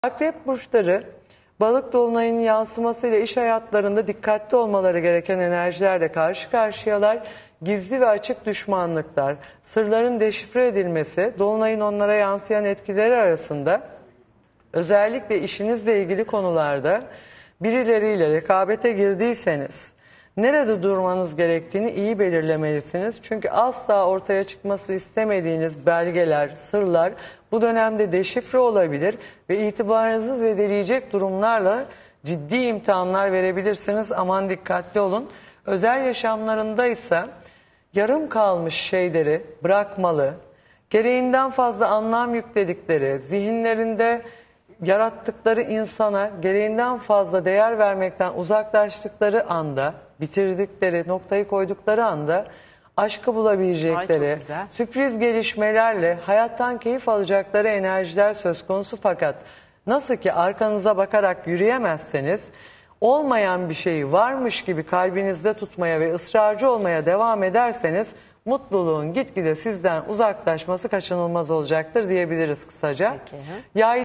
Akrep Burcu | Dolunayda burçları neler bekliyor?
Ebru Akel'in konuğu olan Astrolog Şenay Devi dolunayda Akrep burcunu nelerin beklediğini anlattı.